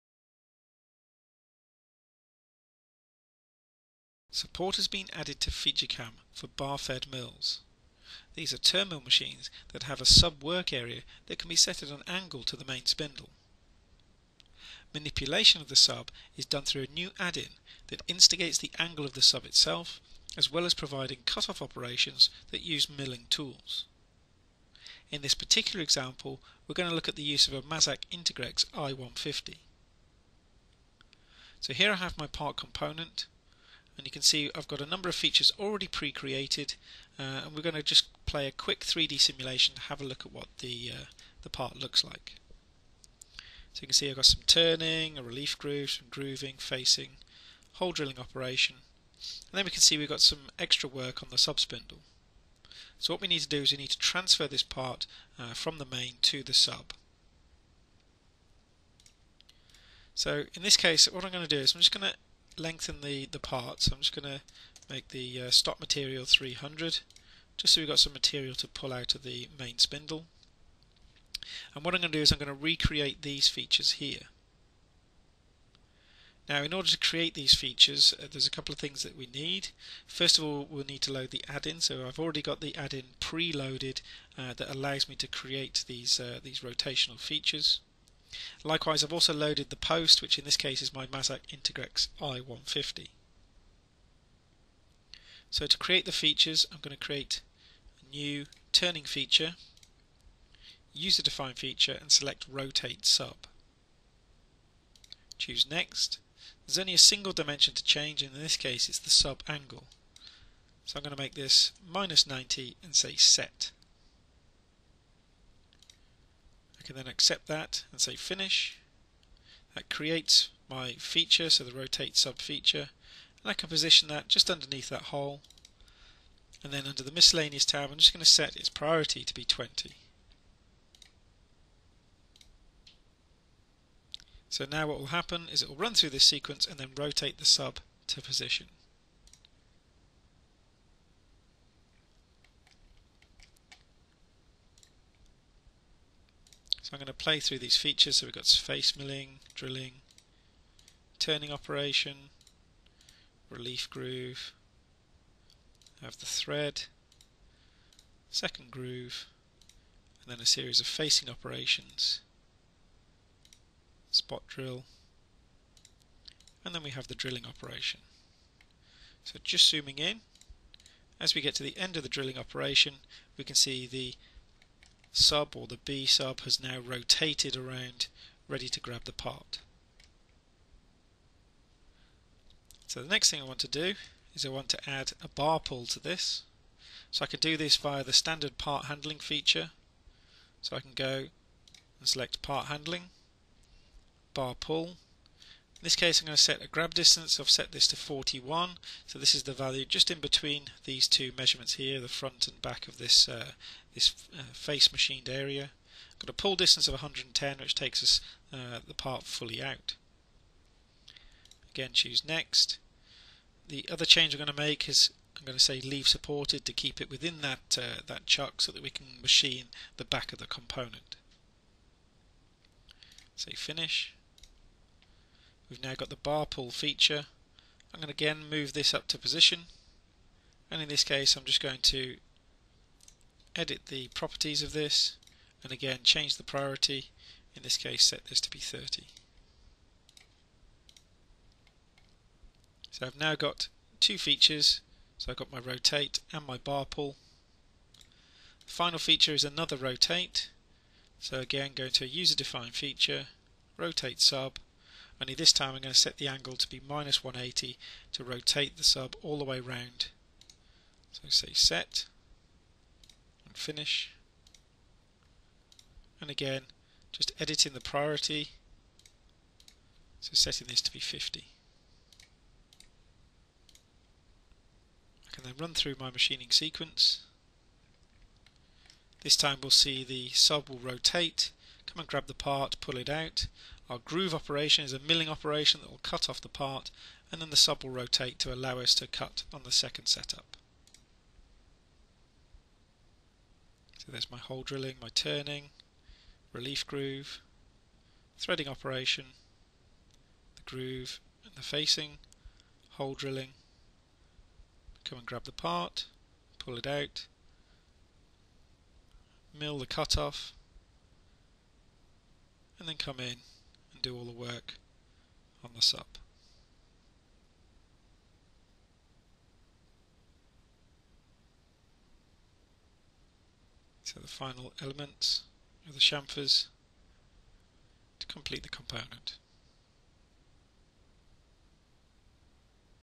Bar Fed Mills - FeatureCAM 2016
Support has been added to FeatureCAM 2016 for multi-tasking bar-fed milling machines.  Access to the back working area is gained with an option to set the swivel angle of the machine, with additional cut-off operations now available that use milling tools.